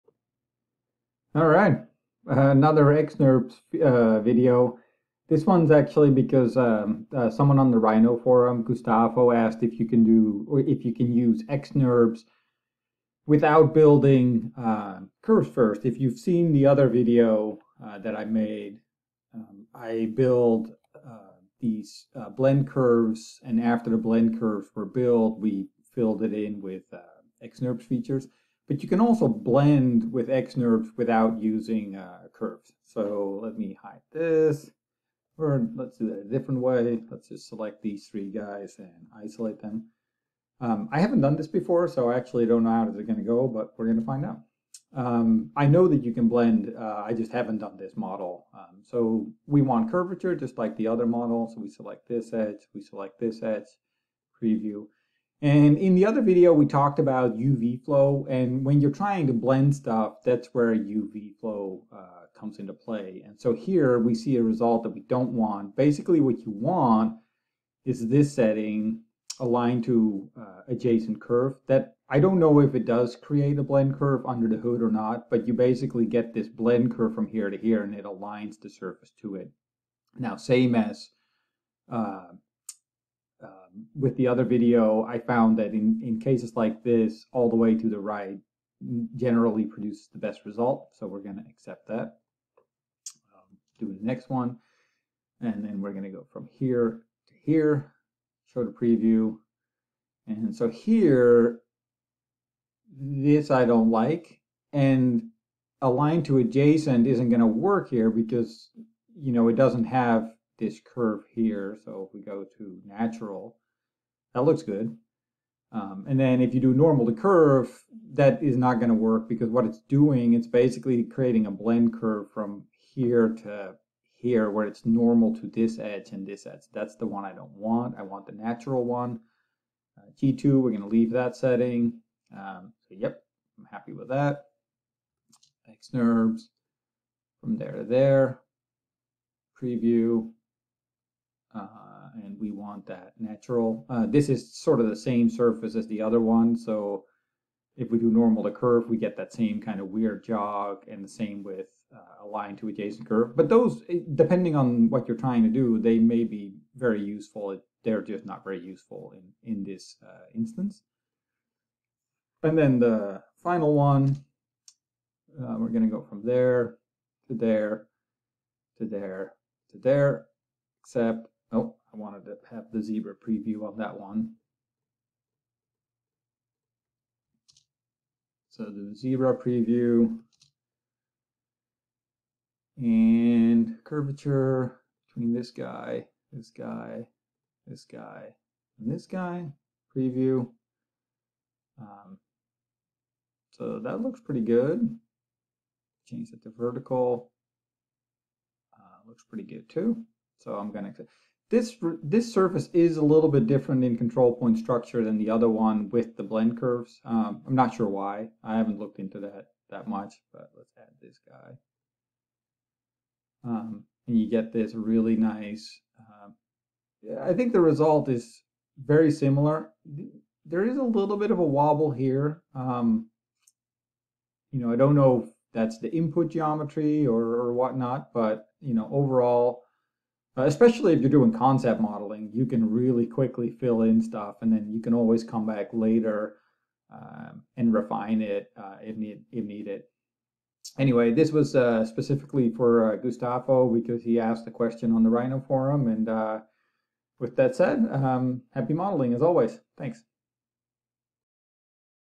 Using XNurbs as BlendSrf for Rhino
Gustavo Fontana, asked on the Rhino forum if you can use XNurbs in the same way as you would use BlendSrf in Rhino. In this video, we blend the same model, but without the help of blend curves.